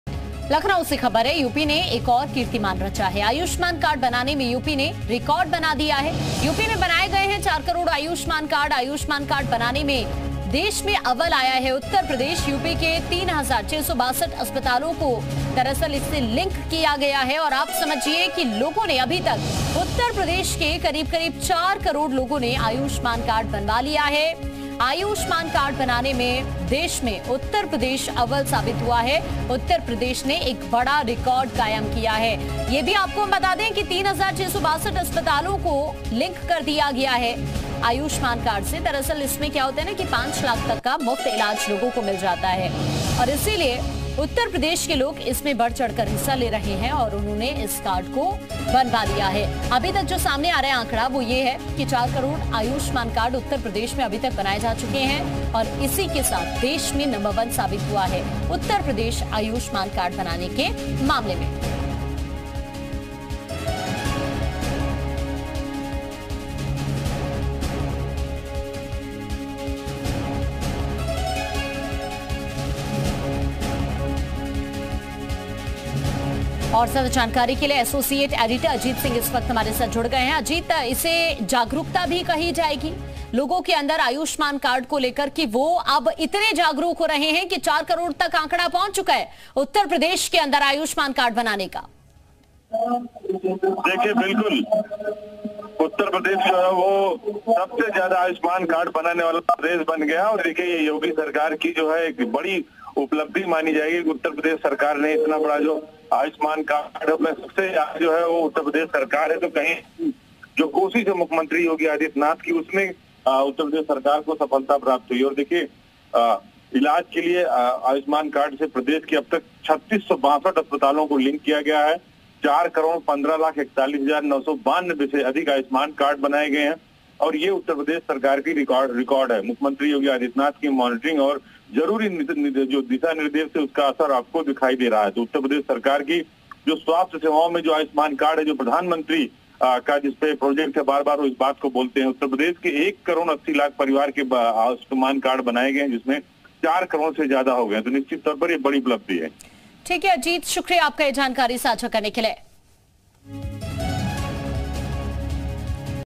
Lucknow : योगीराज में यूपी ने रचा एक और कीर्तिमान, Ayushman Card बनाने में यूपी बना अव्वल | UP News
Lucknow : उत्तर प्रदेश ने रचा एक और कीर्तिमान, आयुष्मान कार्ड बनाने में बनाया रिकॉर्ड, यूपी में बनाए गए 4 करोड़ आयुष्मान कार्ड

Lucknow: Uttar Pradesh created another record, made a record in making Ayushman cards, 4 crore Ayushman cards were made in UP.